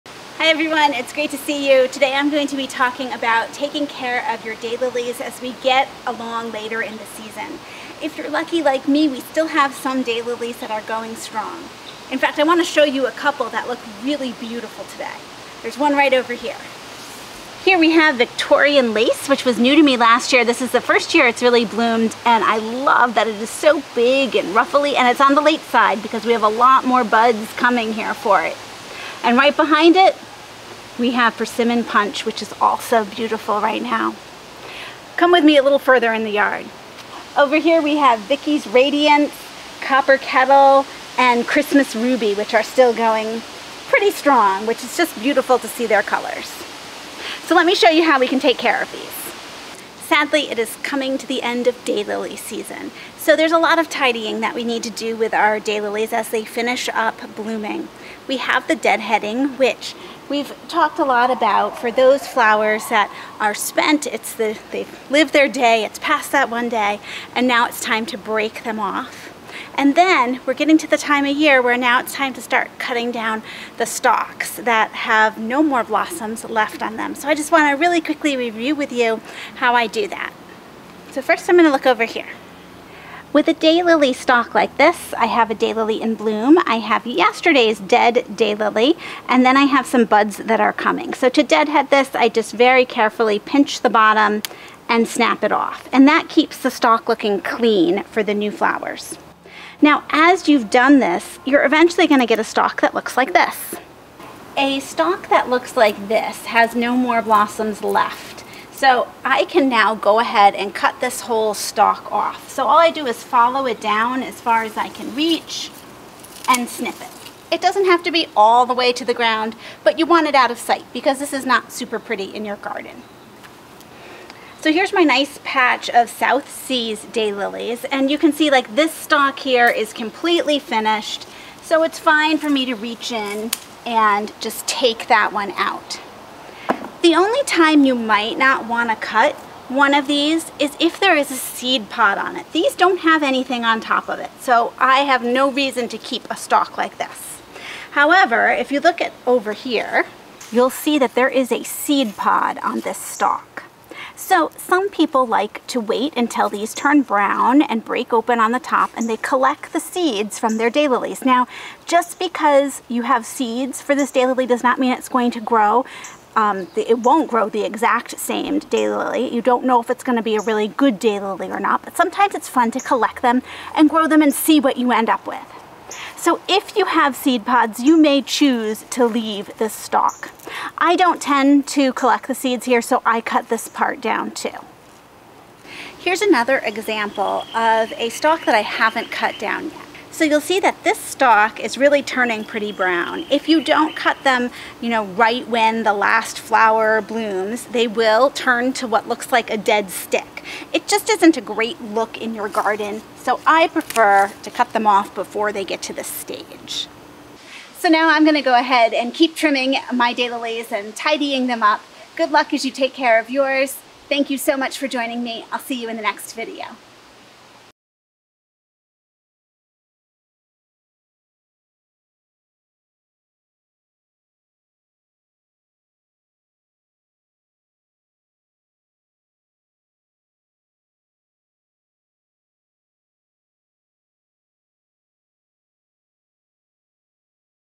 End of Daylily Season
The Garden Mouse tidies up her daylilies at the end of the blooming season.

The Garden Mouse enjoys flower gardening...especially daylilies and hydrangeas.  She is located in the U.S.A. in Zone 6b.